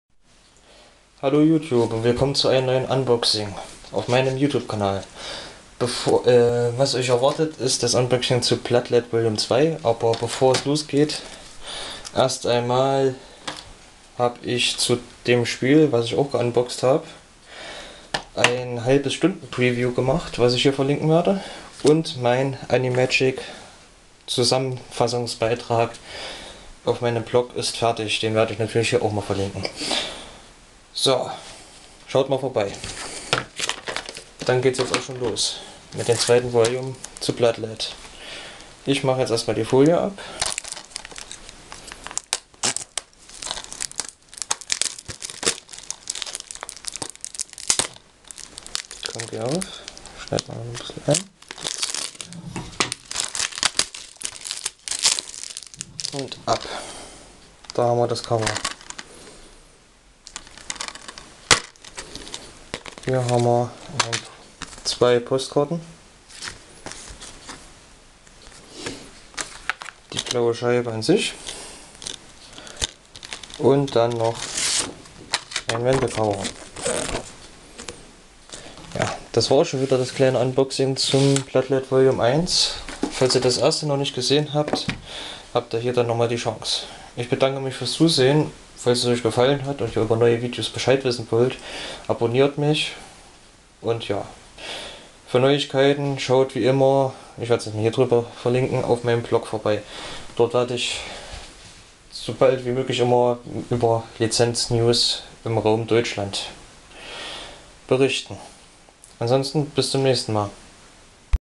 Unboxing Blood Lad Volume 2 [ deutsch - german ]
Das zweite Volume von "Ich bin ein Vampier, will aber nich das machen was Vampiere normalerweise tun" - Blood Lad.